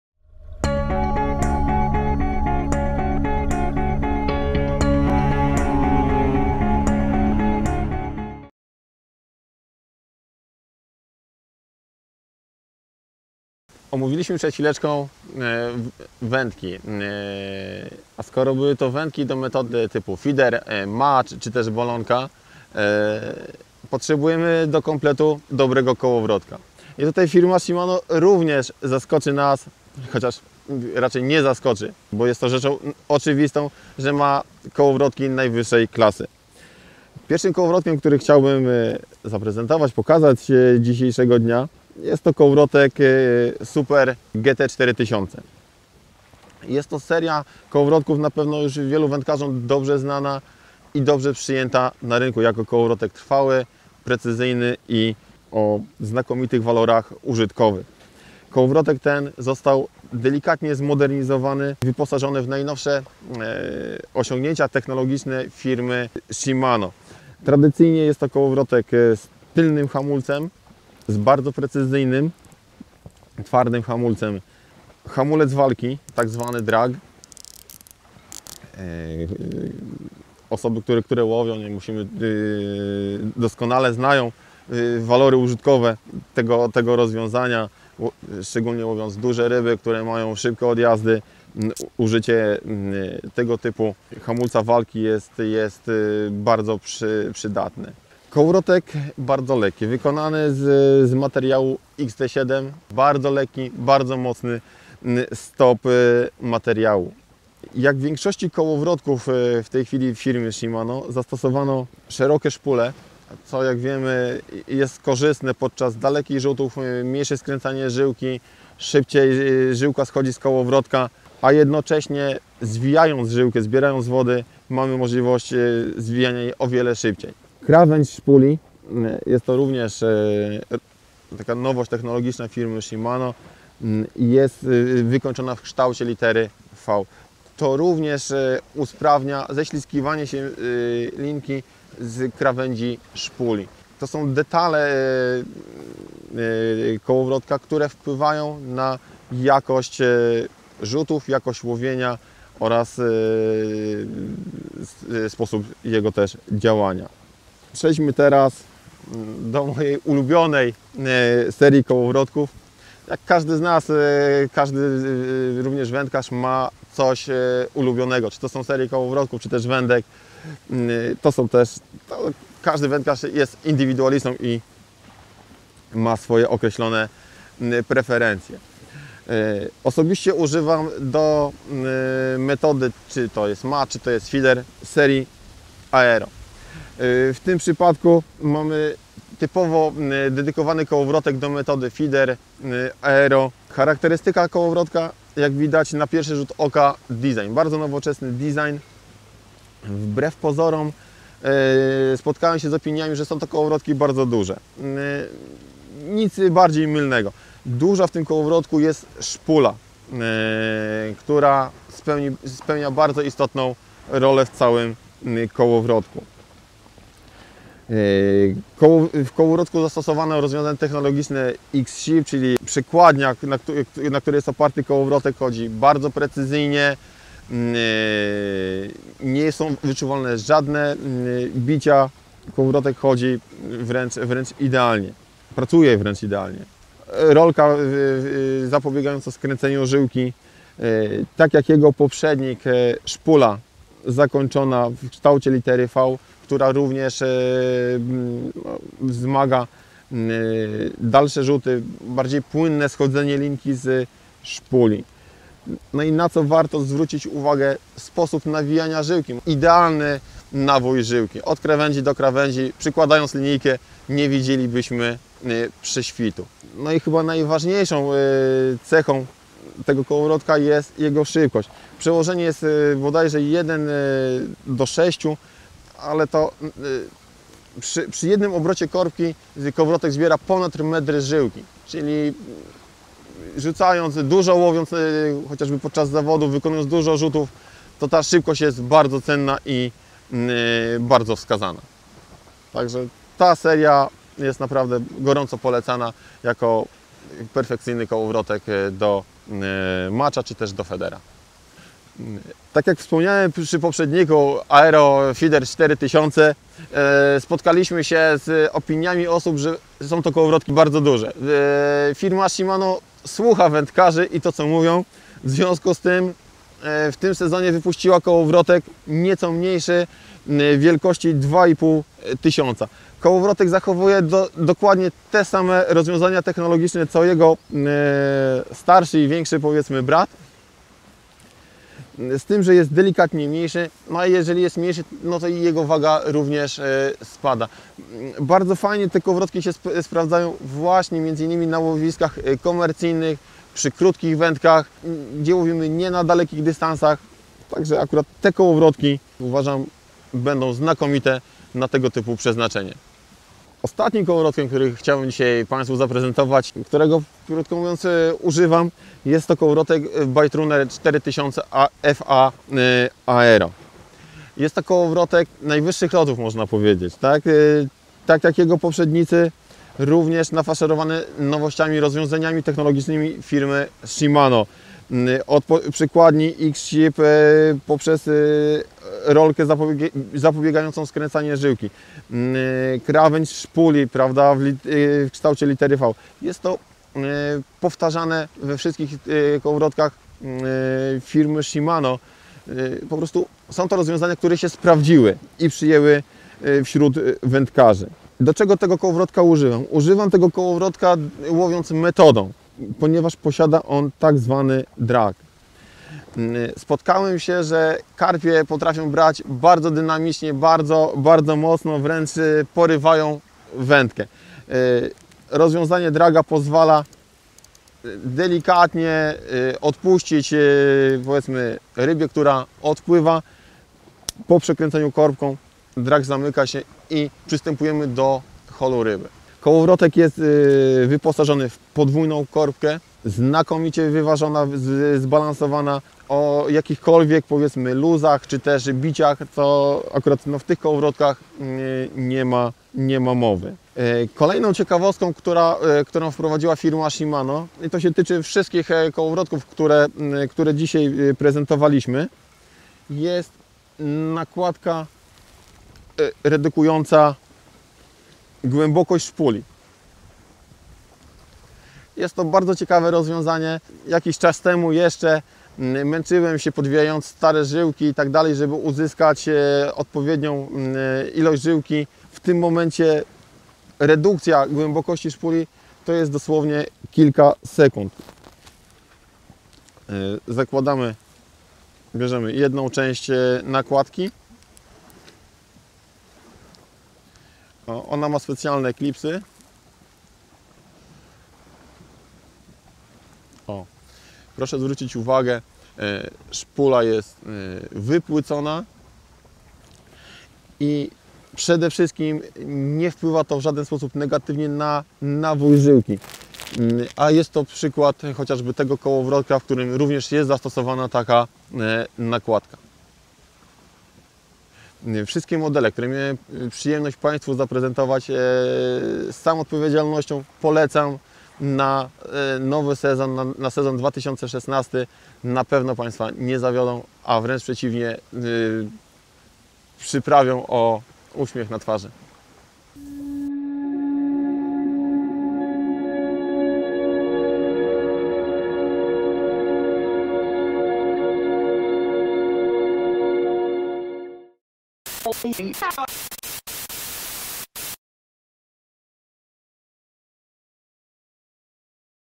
Kołowrotki Shimano 2016
Mariusz Sułek opowiada o kołowrotkach Shimano:
Super Gt 4000
Aero Feeder 4000 oraz Aero Feeder 2500
Baitrunner 4000 FA Aero